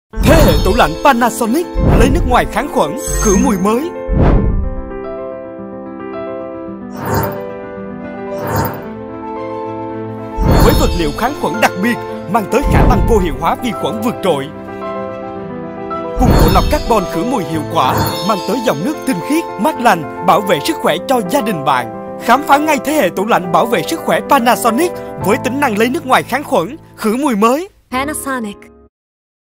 Công Nghệ Lấy Nước Ngoài Kháng Khuẩn Khử Mùi - Trên Thế Hệ Tủ Lạnh Panasonic Mới
DUY NHẤT TẠI PANASONIC DUY NHẤT TỦ LẠNH BẢO VỆ, LẤY NƯỚC NGOÀI KHÁNG KHUẨN - KHỬ MÙI

🌟Được thiết kế cho cả nhà AN TÂM SỐNG KHỎE với dòng nước mát mỗi ngày:

✅Ngăn chứa nước làm VẬT LIỆU KHÁNG KHUẨN BIỆT

✅Tích hợp bộ lọc filter Carbon KHỬ MÙI HIỆU QUẢ trong ngăn chứa nước

✅Tiện lợi với bình chứa nước LÊN tới 3L

Thế hệ tủ lạnh BẢO HIỂM VỆ SINH.